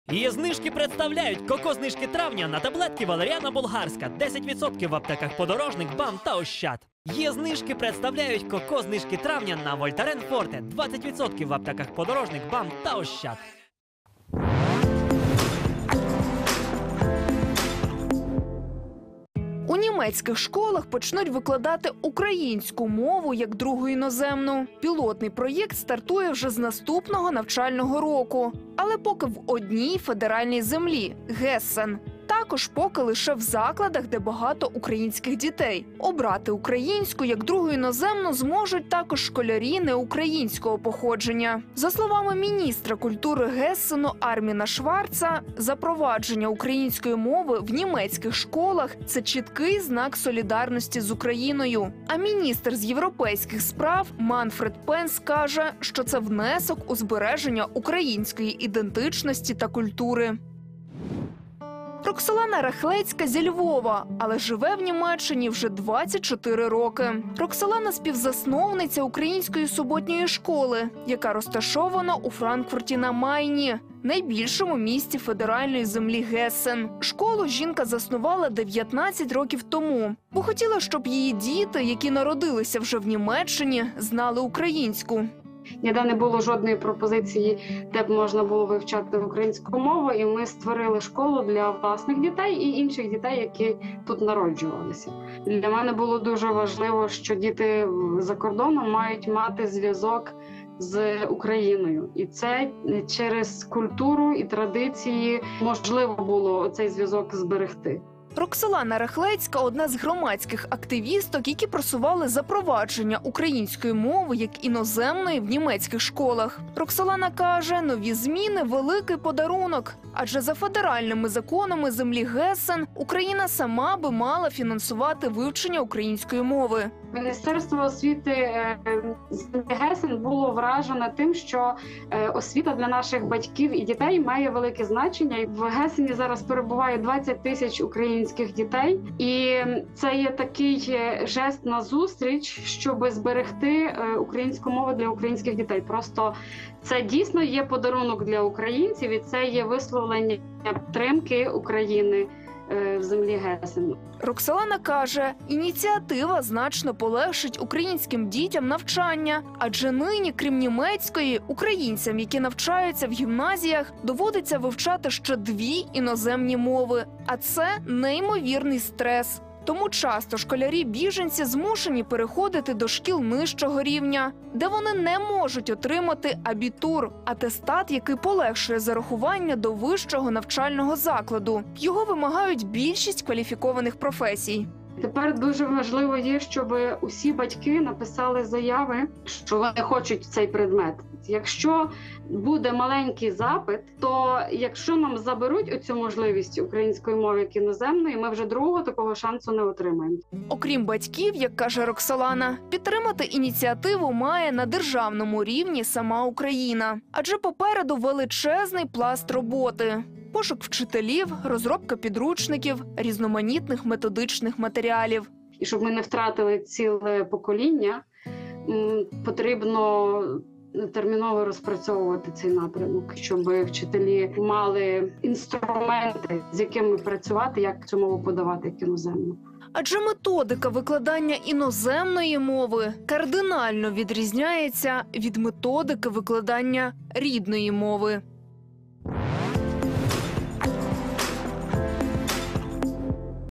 Українська як ДРУГА ІНОЗЕМНА: нововведення у школах Німеччини
У німецьких школах почнуть вивчати українську мову як другу іноземну. Пілотний проєкт стартує у федеральній землі Гессен і має розпочатися вже з наступного навчального року. Детальніше про нововведення та про те, як вони вплинуть на українських школярів-біженців – у нашому сюжеті. Дивімося.